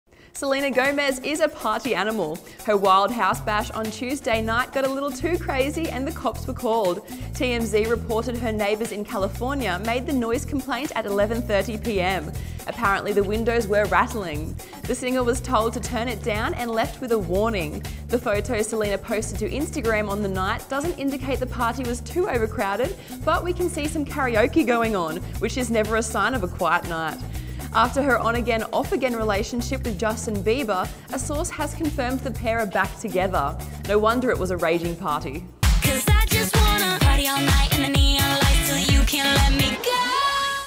Selena Gomez Throws Wild Party
Just how wild was Selena's party? Upbeat Juice investigates ... and is Ms Gomez back together with Justin Bieber? We give our thoughts on the on-off again couple